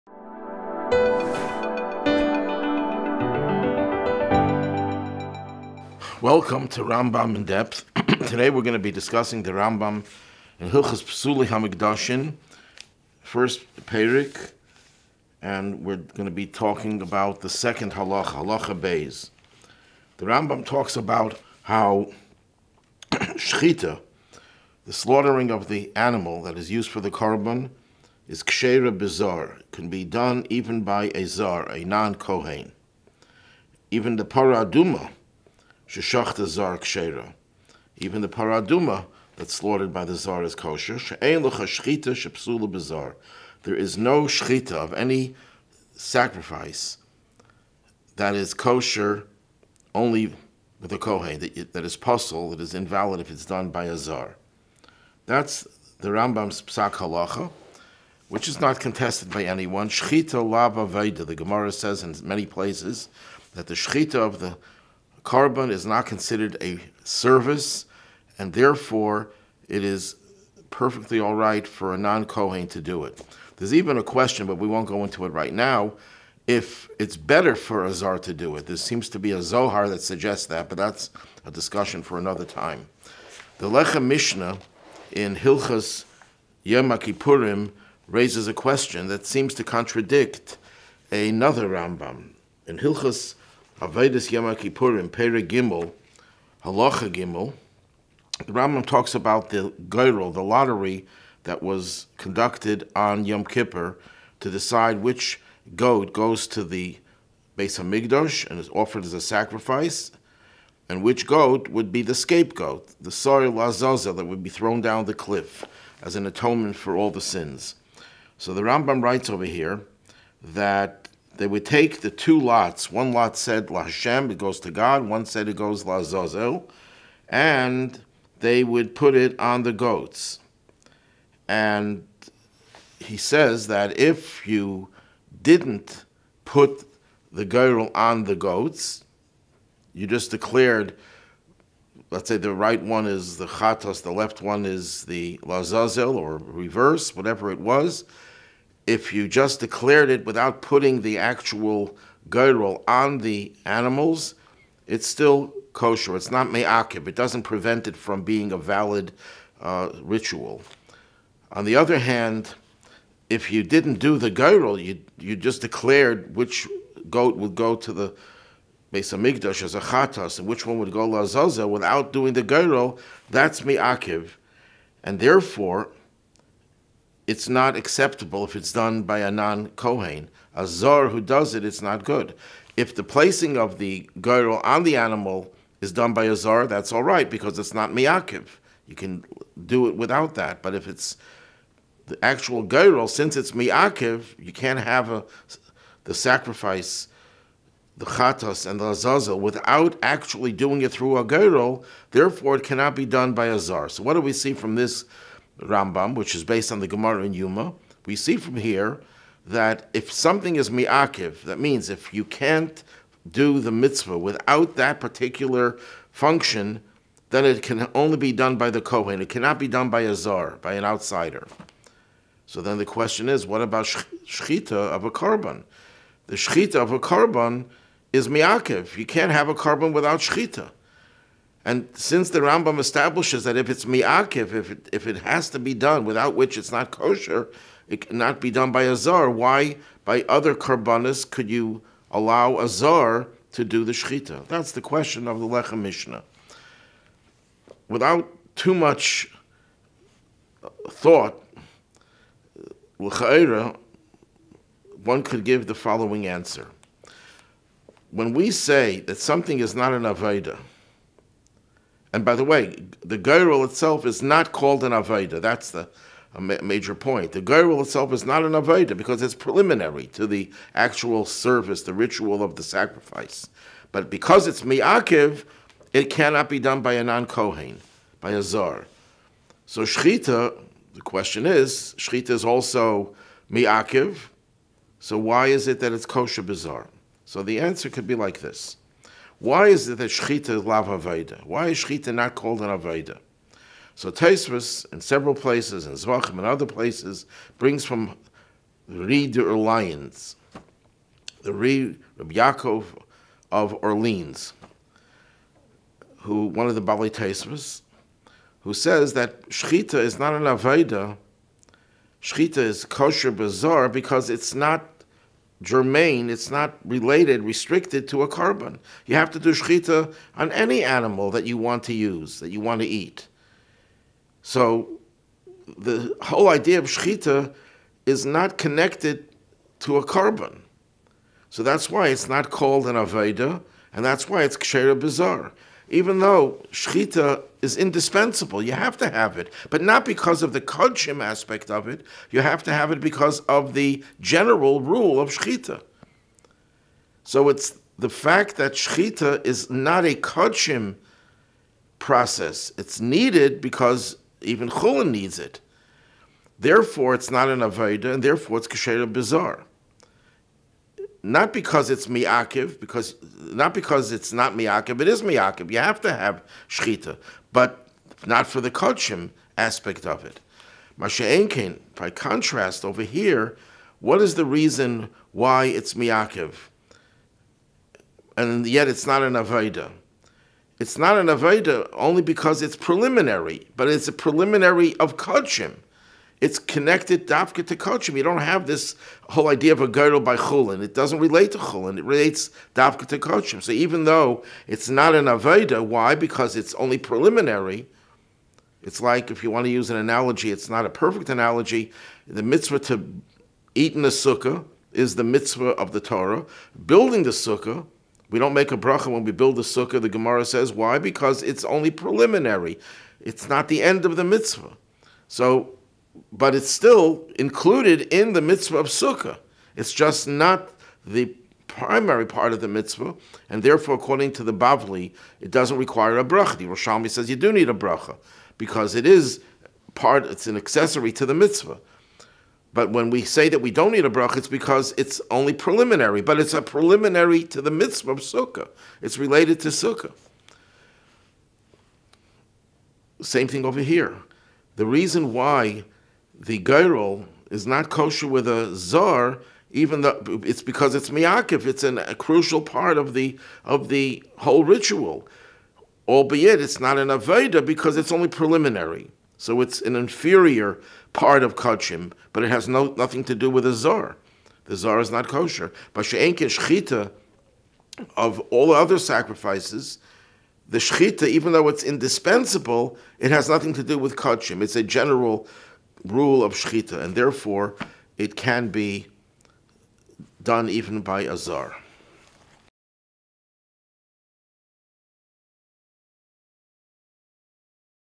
Rambam In-Depth: The Temple Activities Permitted to a Zar (Non-Kohen)
The difference between Shechita and the Goral on Yom Kippur
Rambam Hilchos Psulei Hamukdoshim Chapter 1